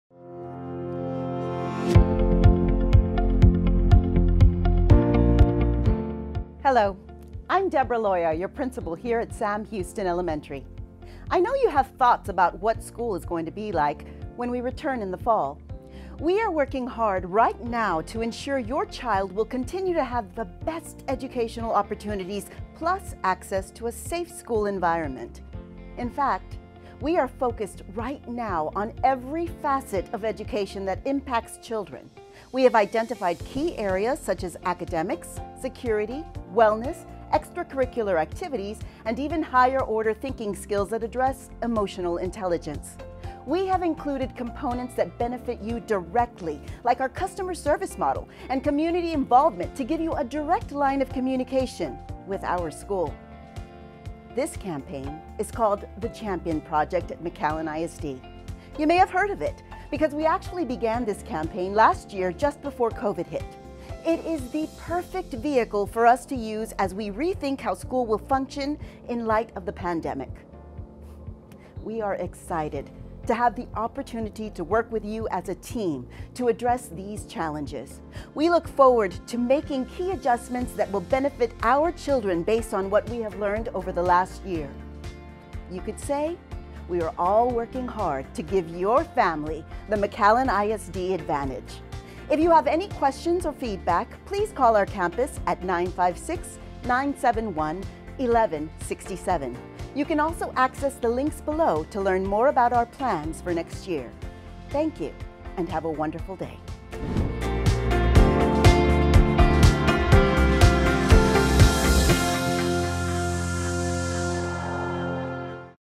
The Champion Project @McAllenISD - Sam Houston Elementary
We know you have thoughts about what school is going to look like when we return in the fall. We are working hard right now to ensure your child will continue to have the best educational opportunities plus access to a safe school environment.

In fact, we are focused right now on every facet of education that impacts children. We have identified key areas such as academics, security, wellness, extracurricular activities and even higher-order thinking skills that address emotional intelligence. We have included components that benefit you directly like our customer service model and community involvement to give you a direct line of communication with our school.

This campaign is called The Champion Project @McAllenISD. You may have heard of it because we actually began this campaign last year just before COVID-19 hit. It is the perfect vehicle for us to use as we rethink how school will function in light of the pandemic.

We are excited to have the opportunity to work with you as a team to address these challenges. We look forward to making key adjustments that will benefit our children based on what we have learned over the last year.

You could say we are all working hard to give your family the McAllen ISD advantage.